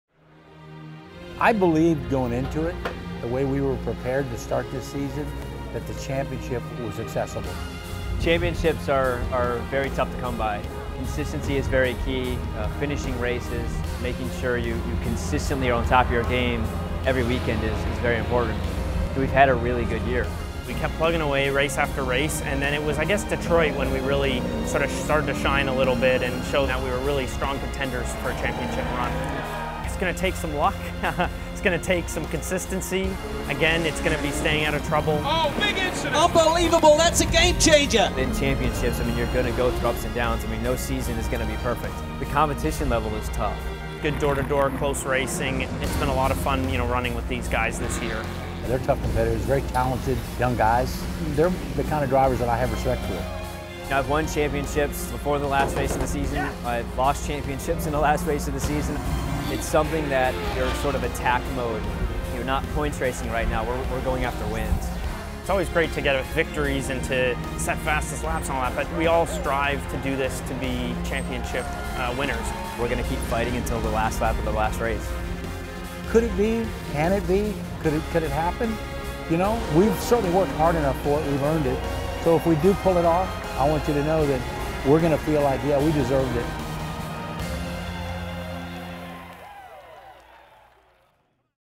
2013 GTS Championship Battle
GTS Championship Battle for 2013 as shown at the 2013 Pirelli World Challenge Banquet